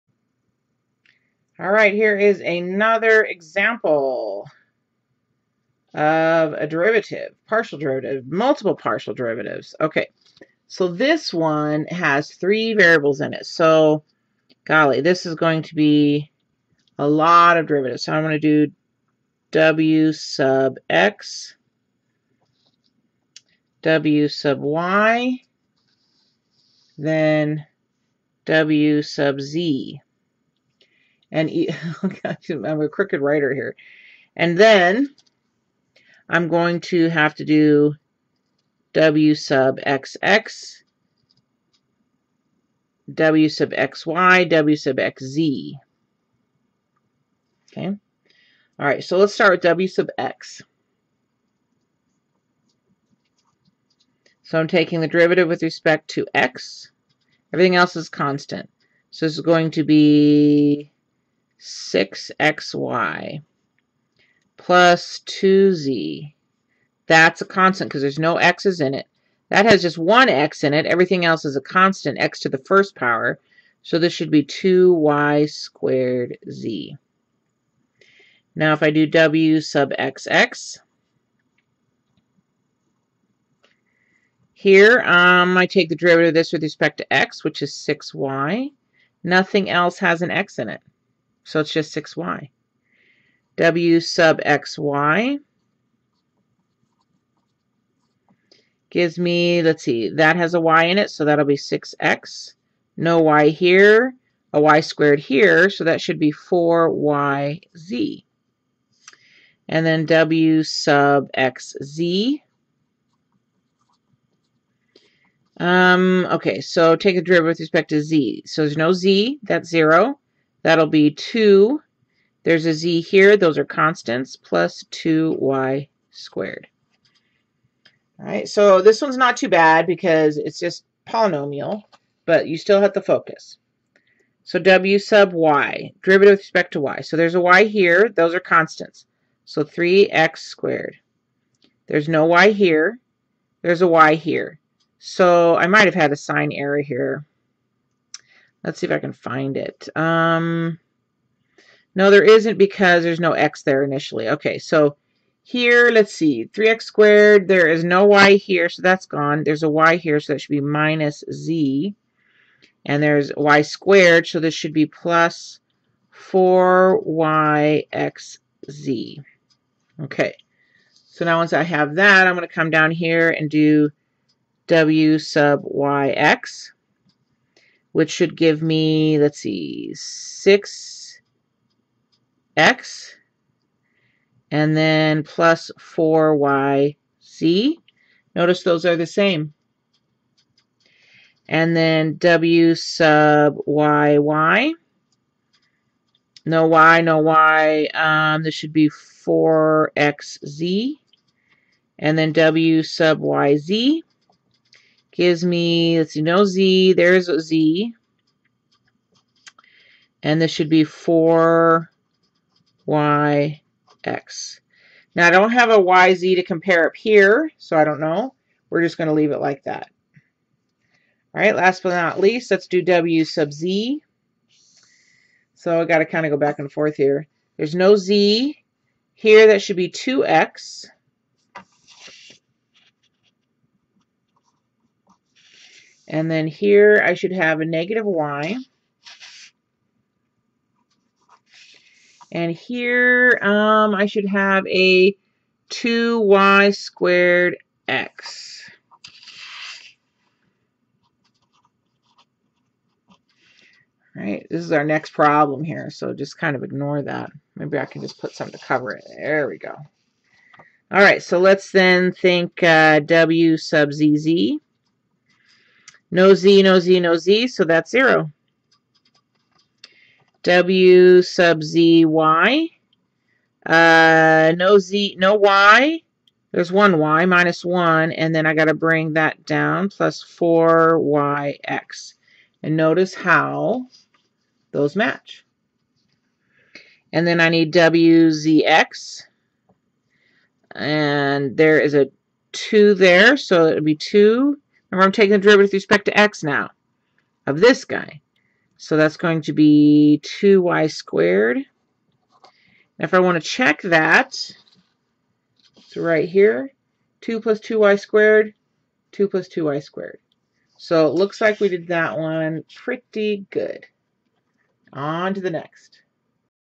Calculus 3 - Second Order Partial Derivatives Part 4
Example calculating second order partial derivatives.